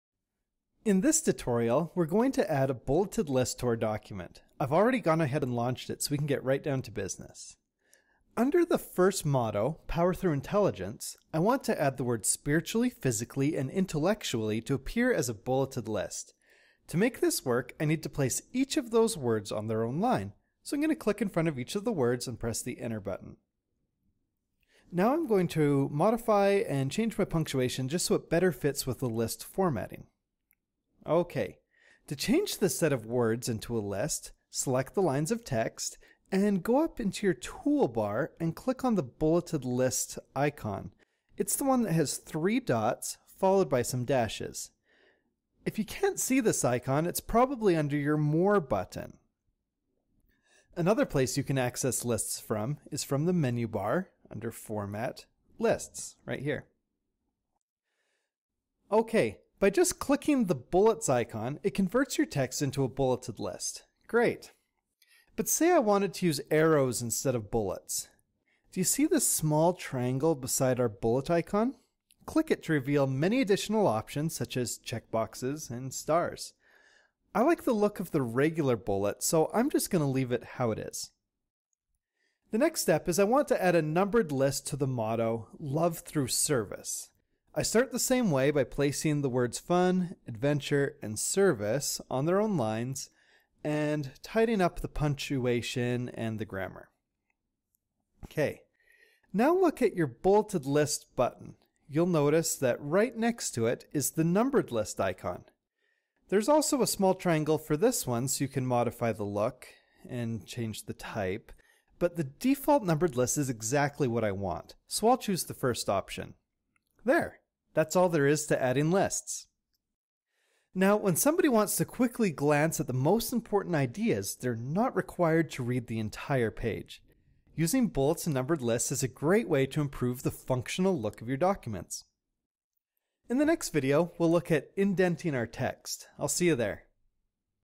How to add Lists & Bullets in Google Docs
In this tutorial we are going to add a bulleted list to our document. I’ve already launched it so we can get right to business.
Under the first motto of Power through Intelligence, I want the words spiritually, physically and intellectually to appear as a bulleted list. To make this work, I need to place each of those words on their own line. So I click in front of each word and press the enter button on my keyboard. I’m also going to remove and modify my punctuation.
To change this set of words into a list, select the lines of text, andI go up to the toolbar and click on the bulleted list icon. It’s the one has 3 dots followed by dashes. If you can’t see this icon, it’s probably under your “more” button.
Another place you can access lists is from the menu bar under format lists.
By just clicking the bullets icon, it converts your text into a bulleted list. But say that I wanted to use arrows in stead of bullets.  Do you see the small triangle beside our bullet icon? Click on it to reveal many additional options such as check boxes and stars. I like the look of the regular bullet, so I’ll leave mine as in.
Next I want to add a numbered list to the motto “Love through Service”. I start the same way by placing the words fun, adventure, and service on their own lines, and tying up the punctuation and grammar.
Now right next to your bulleted list button, you’ll notice the numbered list icon. This also has a small triangle for you to modify the look, but the default numbered list is exactly what I want, so I’ll choose the first option.

There! That’s all there is to adding lists!
Now when someone wants to quickly glance at the most important ideas they’re not required to read the entire page.

Using bullets and numbered lists is a great way to improve the functional look of your documents.
In the next video we will look at indenting our text. See you there!